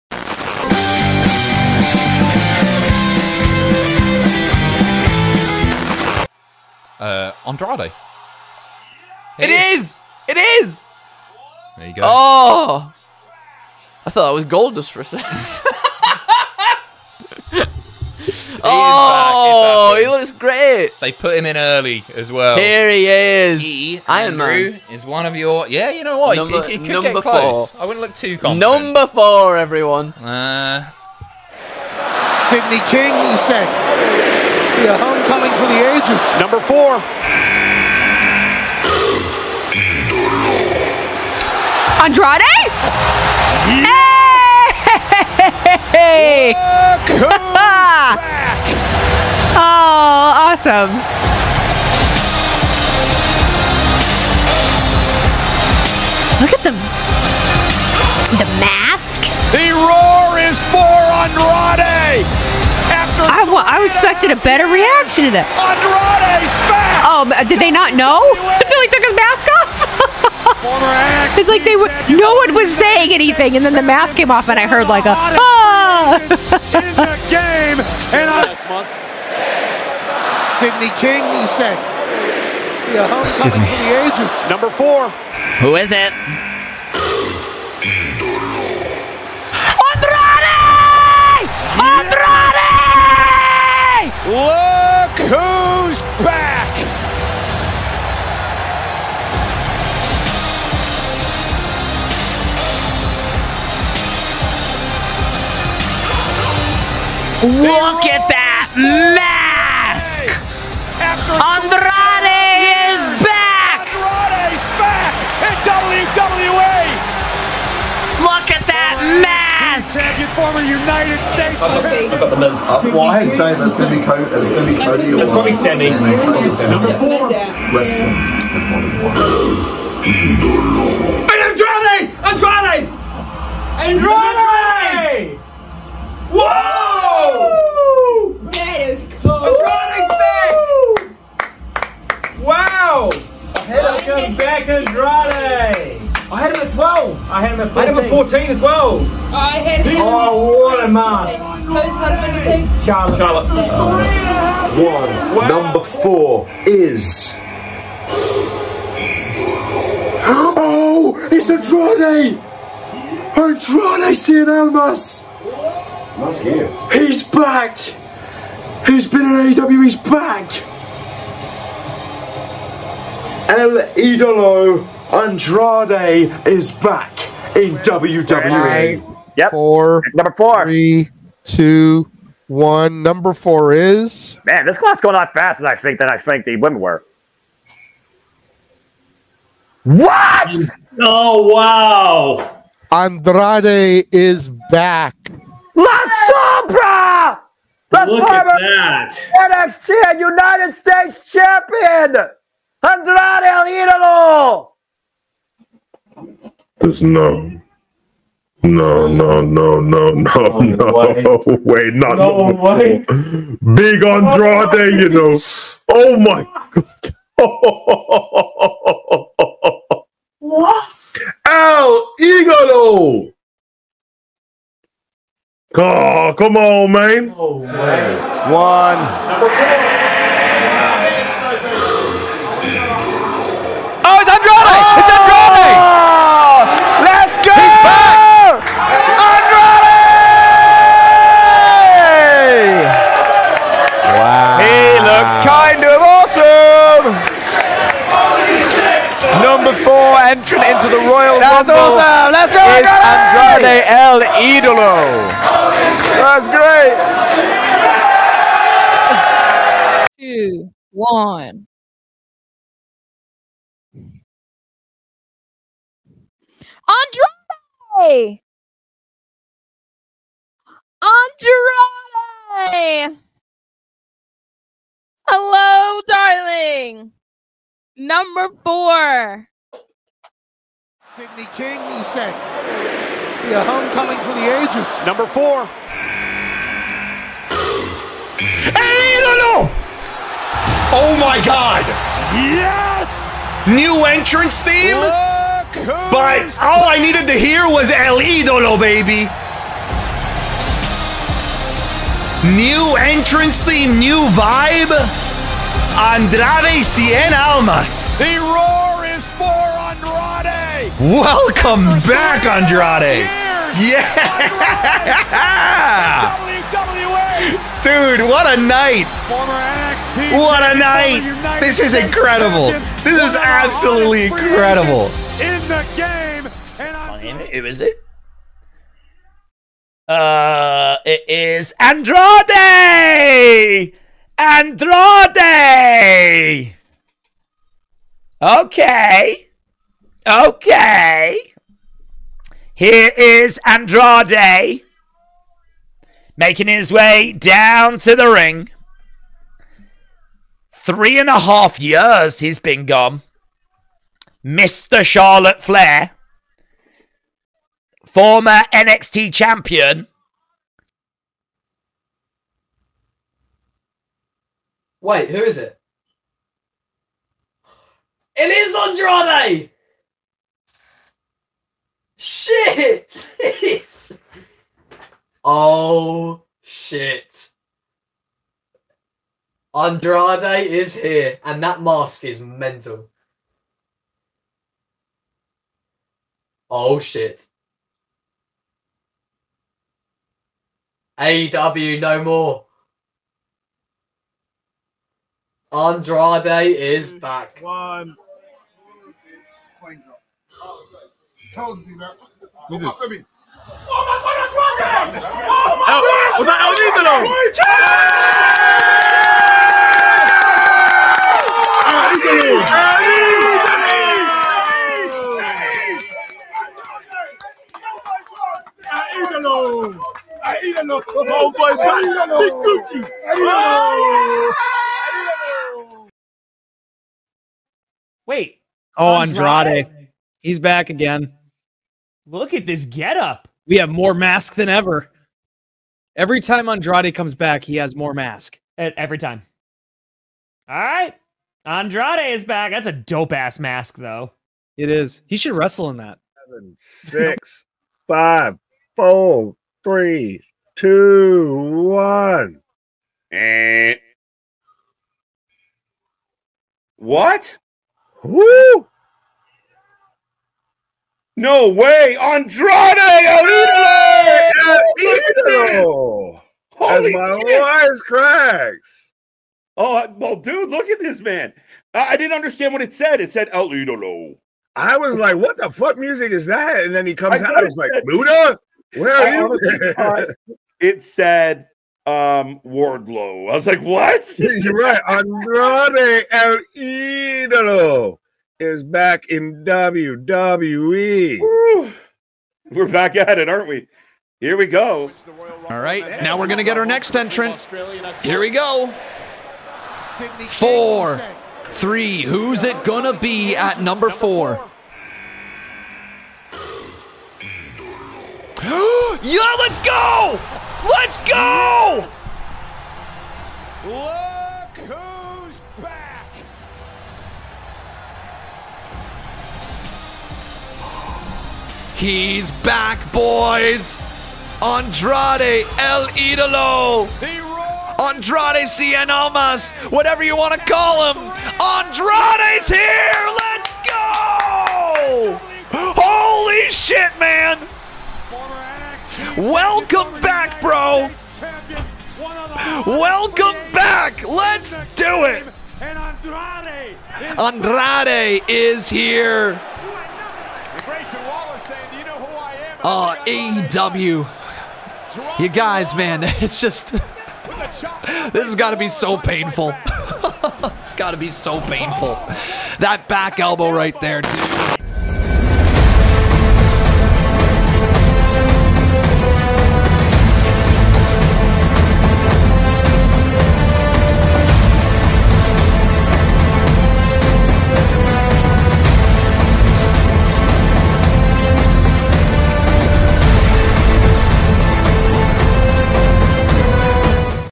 Andrade Returns! Streamers React MEGA collection!!
Andrade makes a much awaited return at the Royal Rumble 2024!
Check out the amazing streamers listed in the video!